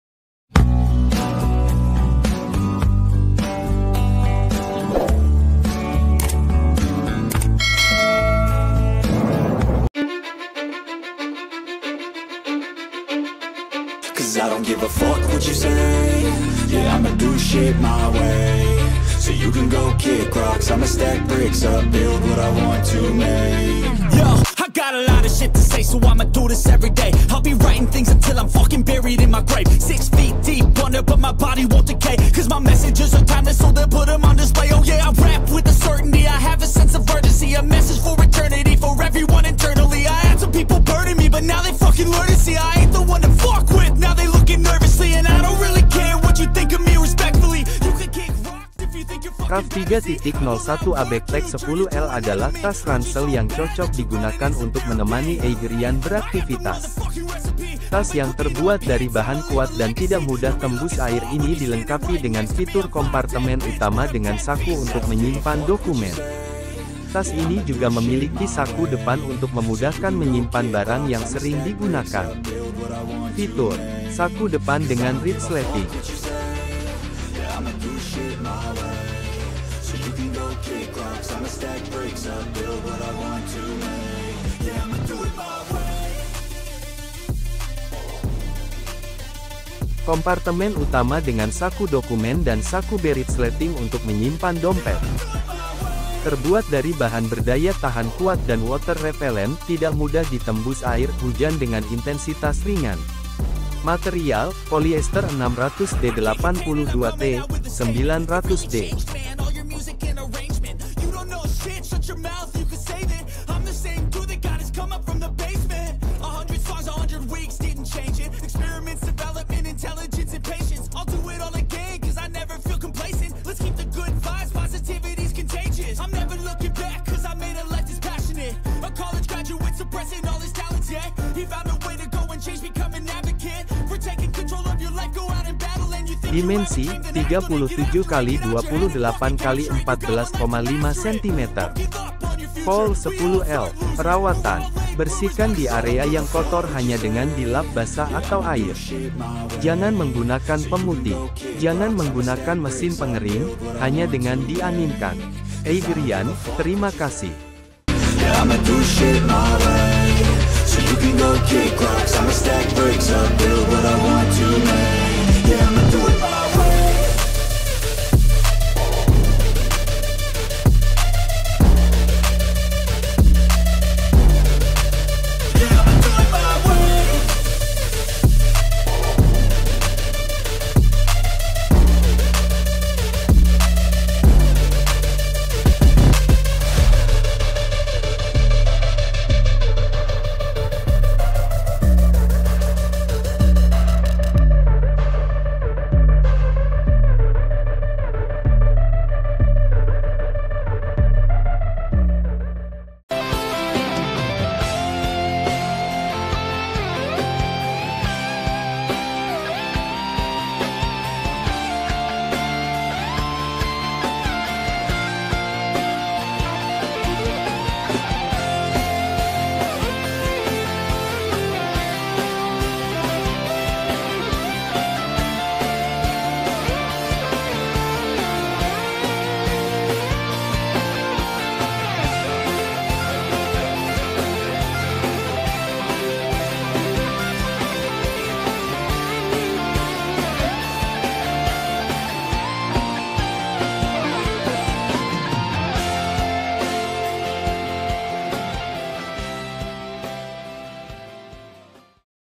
EIGER RAFT 3.0 1A 10L 1989 || 910007934 || NEW VERSION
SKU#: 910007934
EIGER RAFT 3.0 1A 10L 1989
BLUE, MAROON, NAVY
IDR 279.000

Detail produk
Raft 3.0 1a Backpack 10L adalah tas ransel yang cocok digunakan untuk menemani Anda beraktivitas.
Tas yang terbuat dari bahan kuat dan tidak mudah tembus air ini dilengkapi dengan fitur kompartemen utama dengan saku untuk menyimpan dokumen.
Tas ini juga memiliki saku depan untuk memudahkan menyimpan barang yang sering digunakan.

Fitur :
• Dimension : 26 x 12,5 x 37 cm (Vol 10L).
• Material : Polyester 600D 82T, 900D.
• Terbuat dari bahan berdaya tahan kuat dan water repellent (tidak mudah ditembus air/hujan dengan intensitas ringan).
• Kompartemen utama dengan saku dokumen dan saku berritsleting untuk menyimpan dompet.
• Saku depan dengan ritsleting.
• Weight : 0.300000.
• Activity : 1989.
• Gender : ALL.

√ • PERAWATAN :
- Disarankan mengunakan air dengan PH rendah,seperti sabun bayi.

|| • PERHATIAN • ||
EIGERIAN, SEMOGA SEHAT SELALU.
TERIMAKASIH.